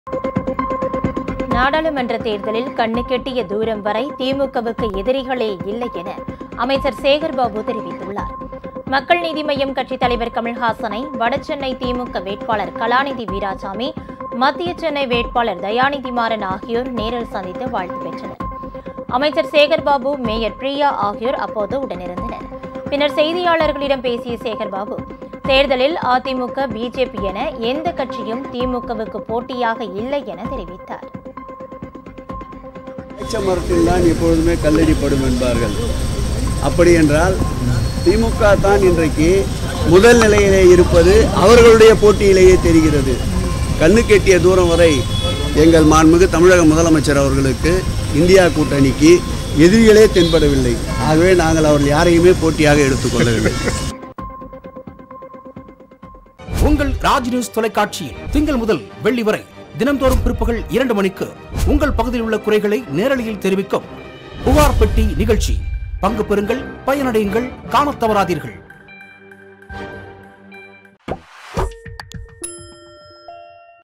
I.N.D.I.A கூட்டணிக்கு எதிரிகளே இல்லை.. அமைச்சர் சேகர் பாபு | INDIAalliance | Minister | ShekharBabu
I.N.D.I.A கூட்டணிக்கு எதிரிகளே இல்லை.. அமைச்சர் சேகர் பாபு